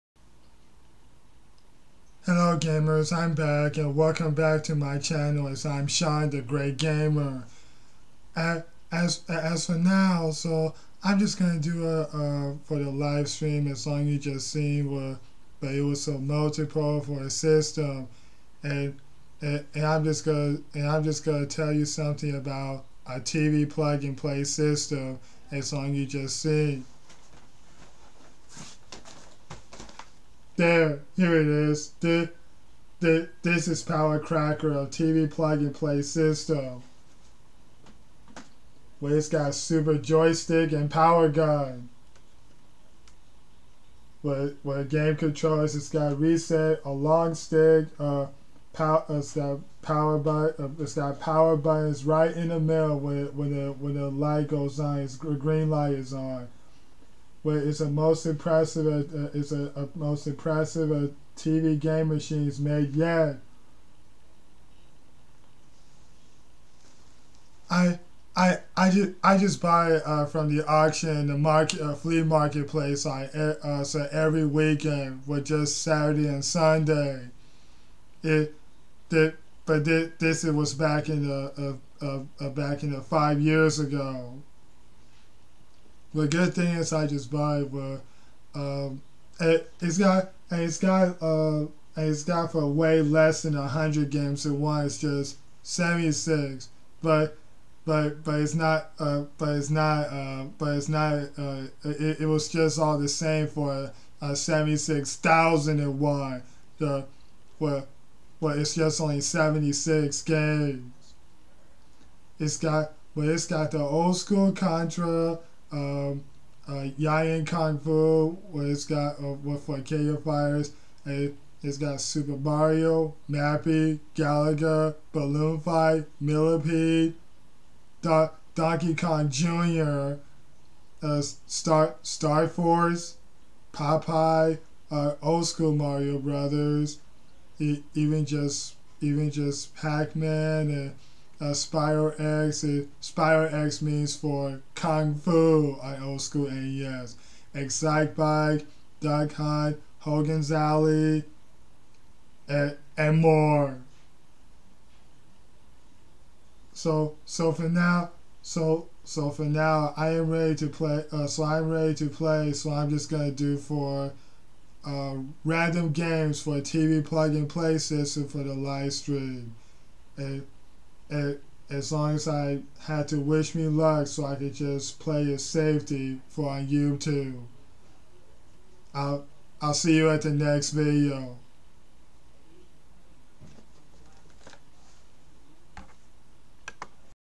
Shawne's Showing The TV Plug & Play System.
The TV Plug & Play system is the Super Joystick & Power Gun. And it's got less than 100 games in 1. just 76 games.

Not just 76,000. At least it's got the old school NES.